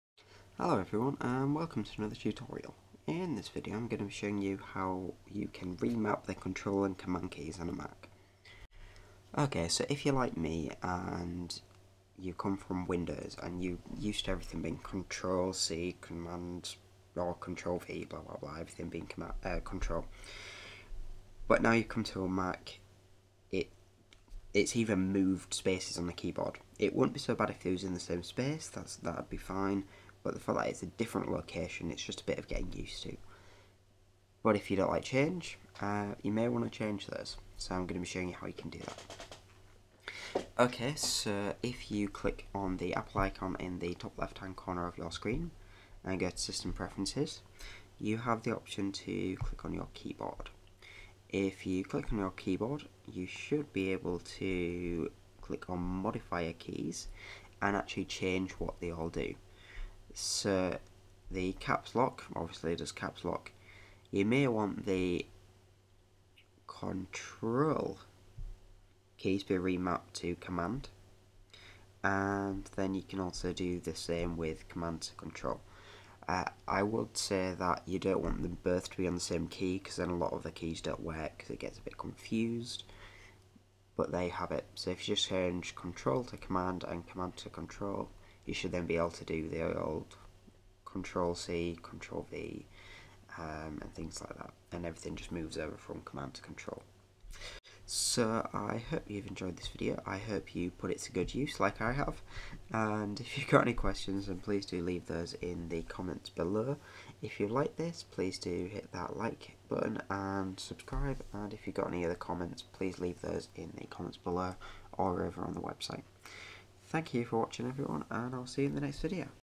How to: Swap the Cmd and Ctrl key on a Mac
In this video, I will be showing you how you can swap the Command and Control Key on a mac. This will allow you to go back to the windows shortcuts such as Ctrl + C, Ctrl +  Z etc.